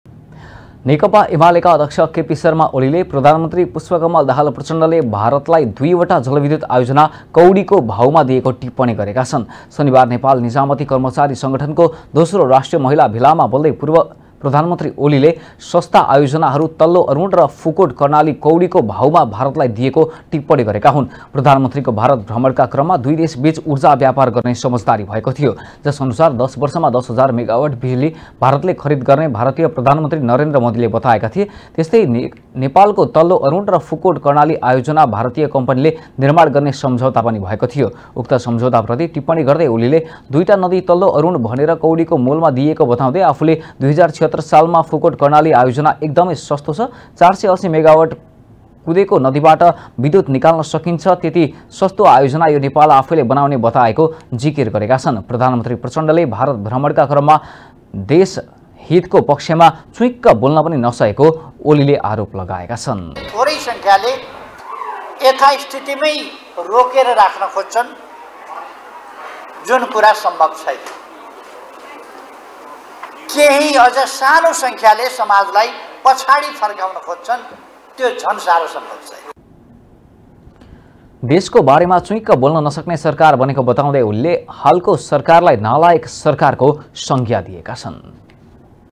प्रधानमन्त्री पुष्पकमल दाहाल प्रचण्डले भारतलाई दुईवटा जलविद्युत आयोजना कौडीको भाउमा दिएको टिप्पणी
नेकपा एमालेका अध्यक्ष केपी शर्मा ओलीले प्रधानमन्त्री पुष्पकमल दाहाल प्रचण्डले भारतलाई दुईवटा जलविद्युत आयोजना कौडीको भाउमा दिएको टिप्पणी गरेका छन् ।